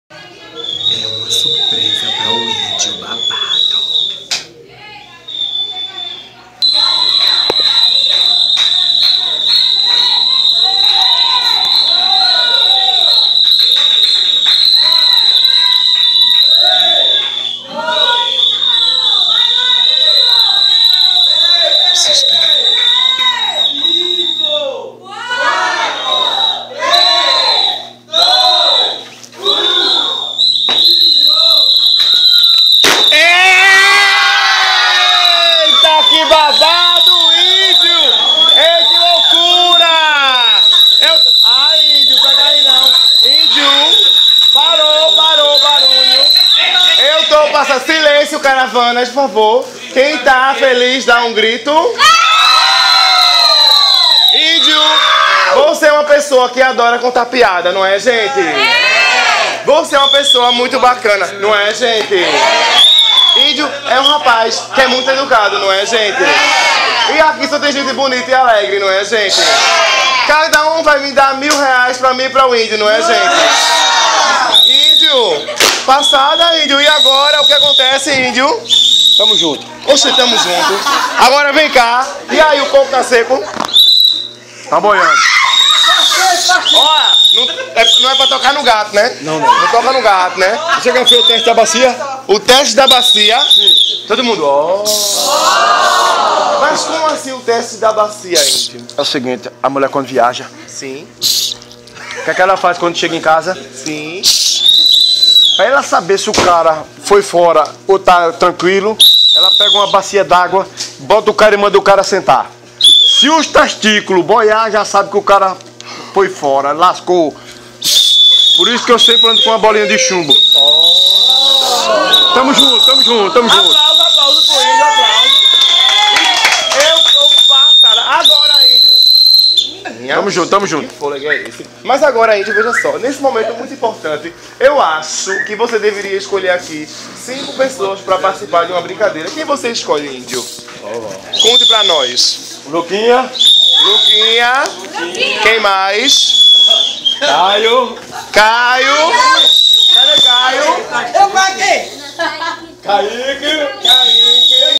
Telegrama Animado com Dragqueen em São José da Coroa Grande 2013 09 07 17 13 54
Praia, Bronze, Verão, 7 de Setembro e Aniversário do Índio meu bem, para saber mais sobre este lele bafaum aqui, Acesse

Bjsss de luz a todos!

Festa e Eventos com Drag Queen
Falar c/: Deyse Cristina / Rafael Silva